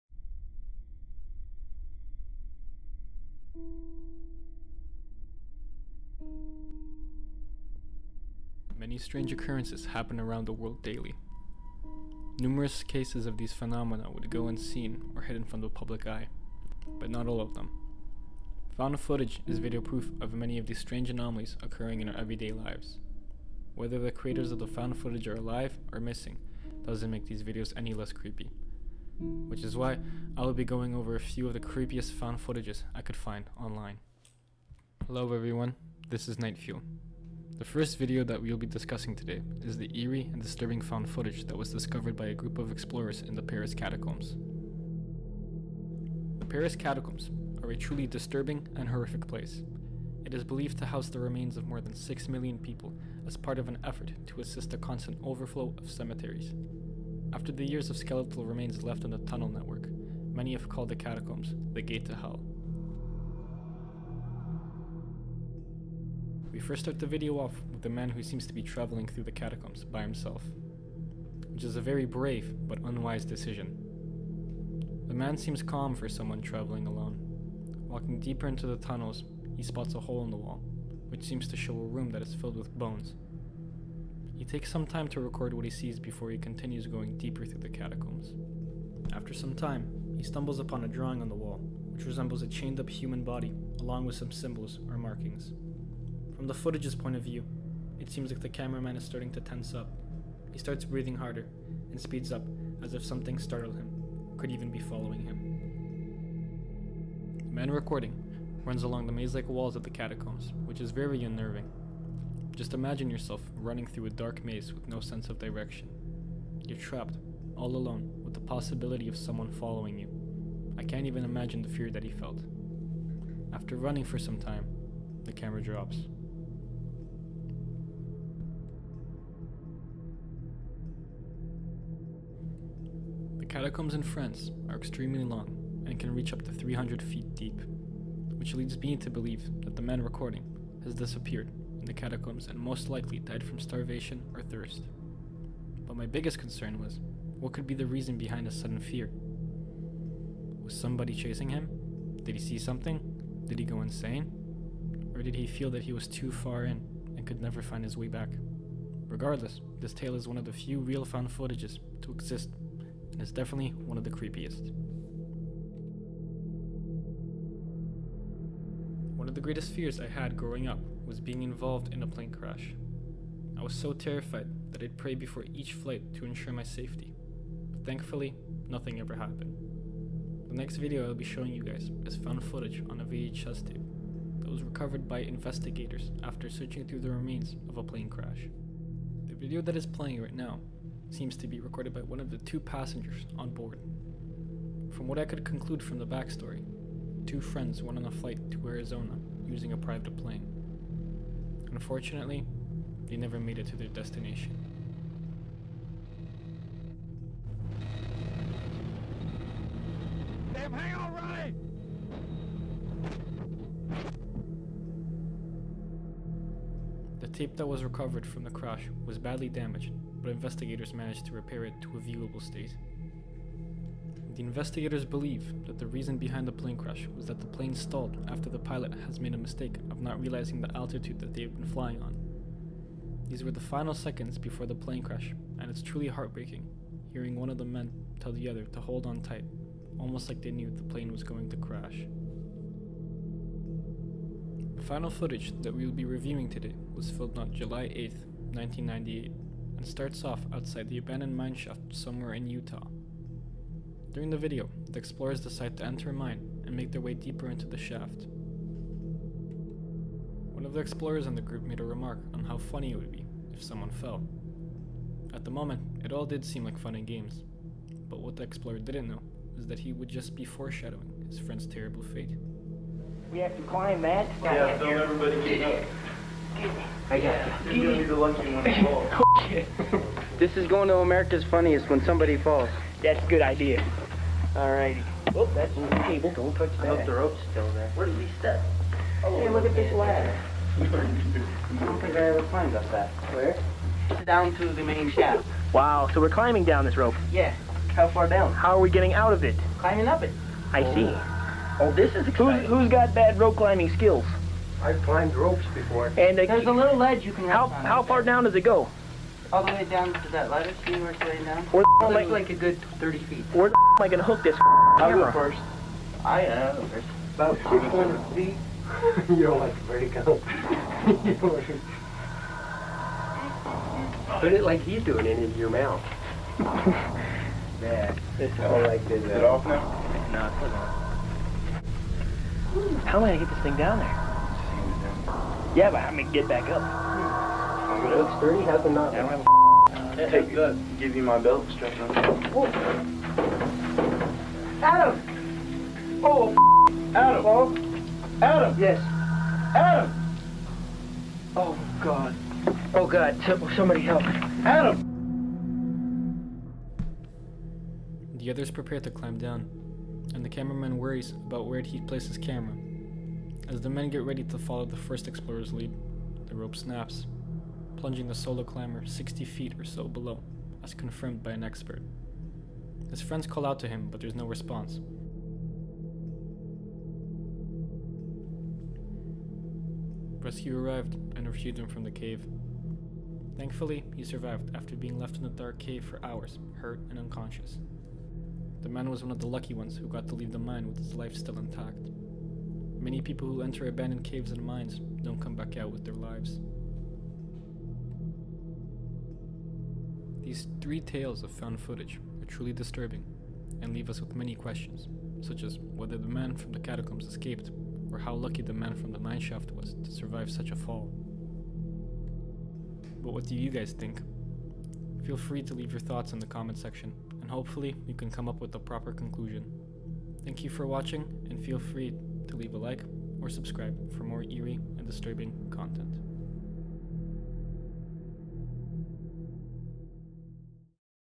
Creepiest “Real” Found Footage
Found footage is very hard to come across nowadays. But when we do find instances of it. We make sure to let it be known. Watch my video to see three cases of creepy found footage. BEWARE